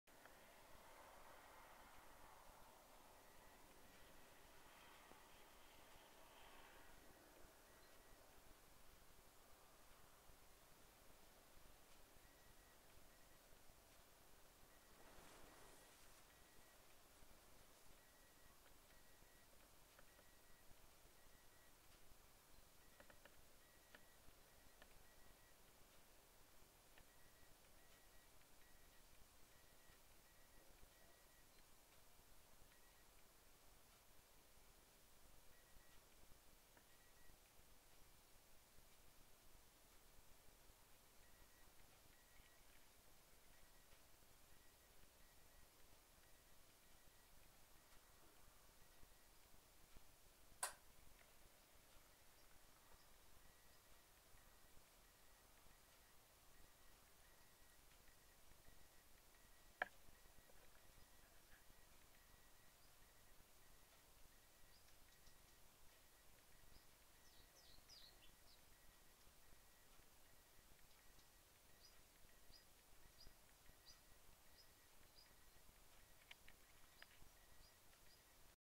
Ekorn.
morsomt å se ekorn beite rett utfor kjøkkenvinduet. fin frokost underholdning og en  flott naturopplevelse. :-)